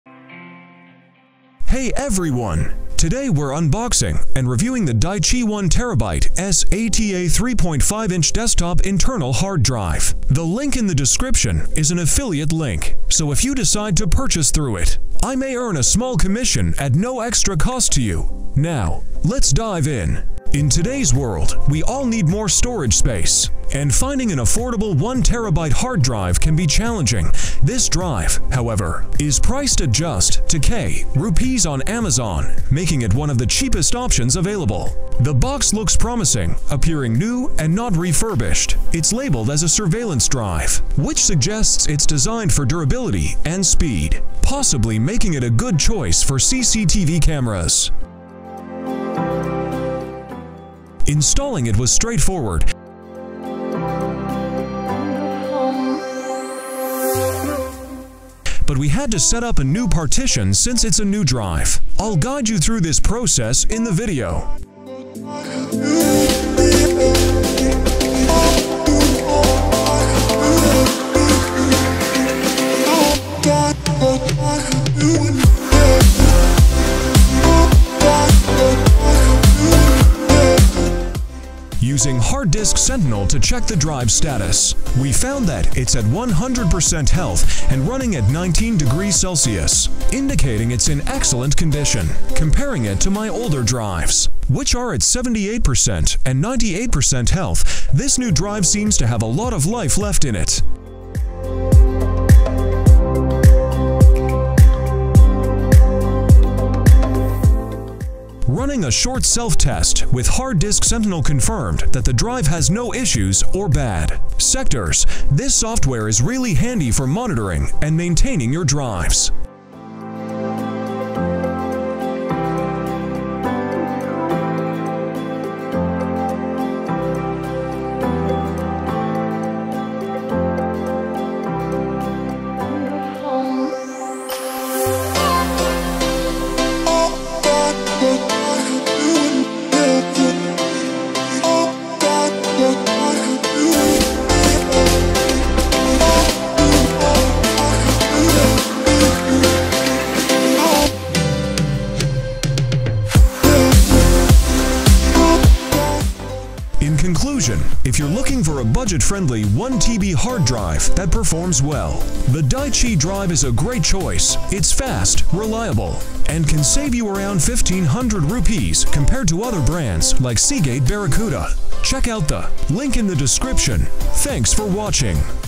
Is the Daichi 1 TB SATA 3.5 Inch Desktop Internal Hard Drive Worth Your Money Unboxing and Review!
Welcome back, tech enthusiasts! Today, we have an exciting unboxing and review of the Daichi 1 TB SATA 3.5 Inch Desktop Internal Hard Drive. In this comprehensive video, we'll delve into the nitty-gritty details of this budget-friendly storage solution to help you make an informed decision.

In a world where digital storage is a necessity, finding an affordable yet reliable 1 TB hard drive can be challenging. The Daichi 1 TB SATA 3.5 Inch Desktop Internal Hard Drive offers a compelling option, priced at just 2000 rupees on Amazon. This makes it one of the most cost-effective choices in its category.

The initial impressions of the packaging are positive. The box looks brand new, not refurbished, which is often a concern with budget-friendly options. The labeling indicates that it's a surveillance drive, hinting at its potential use in CCTV systems. This suggests a focus on durability and reliability, which are crucial factors in a hard drive intended for constant use.

Upon unboxing, we find the hard drive well-protected with standard packaging materials. Installation is straightforward, although setting up a new partition is required since it's a new drive. Don't worry if you're unfamiliar with the process; I'll guide you through it step by step in this video.

Using Hard Disk Sentinel to check the drive's health and performance, we discover that it's in excellent condition, running at 100% health and 23 degrees Celsius. This indicates that the drive is indeed new and has not been used extensively.

In conclusion, if you're in the market for a budget-friendly 1 TB hard drive that delivers on performance and reliability, the Daichi drive is a compelling choice. Its affordability, coupled with its promising features, make it a standout option in its price range.